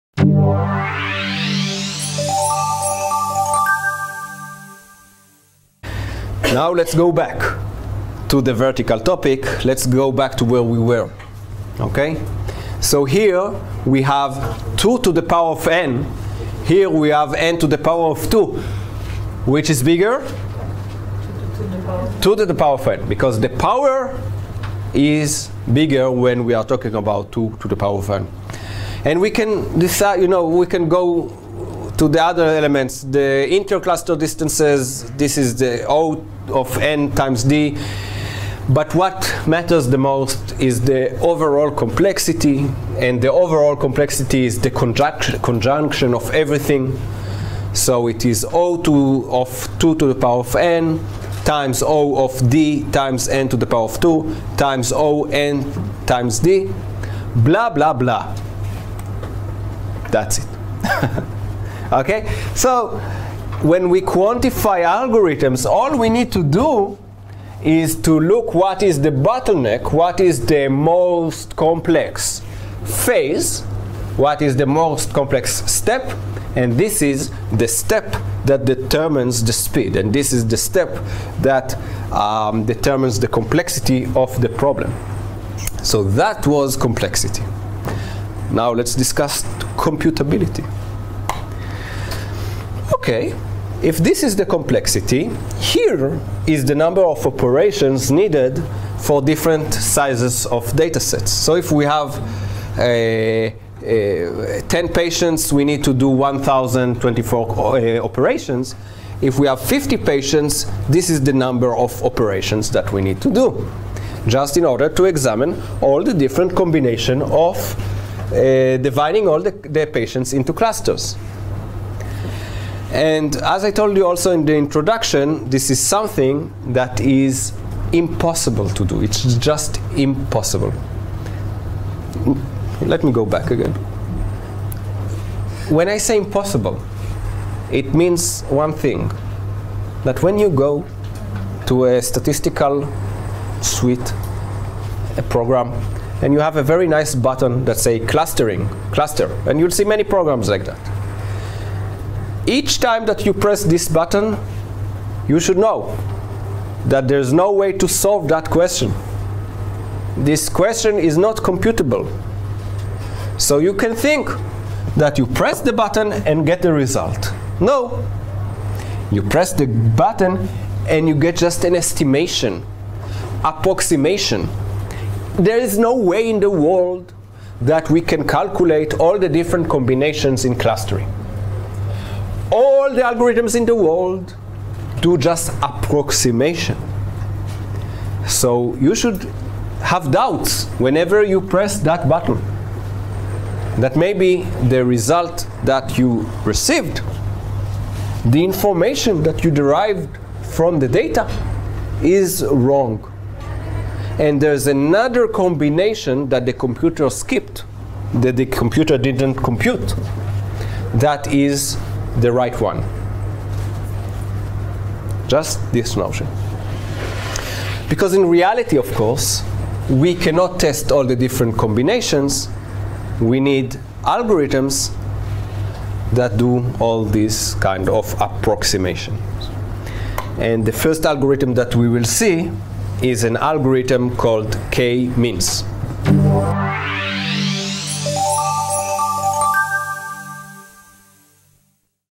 Quantification of the Clustering Problem - Part 2 | Medical Data Mining L02T09C | Unsupervised ML.
The Online Certificate Program in
Genomics and Biomedical Informatics

Bar-Ilan University & Sheba Medical Center

Course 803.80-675 - Medical Data Mining
Spring 2018

Lecturer: Dr. Ronen Tal-Botzer

Unit L02: Unsupervised Machine Learning | From Data to Information to Knowledge
Topic 09C: Quantification of the Clustering Problem - Part 2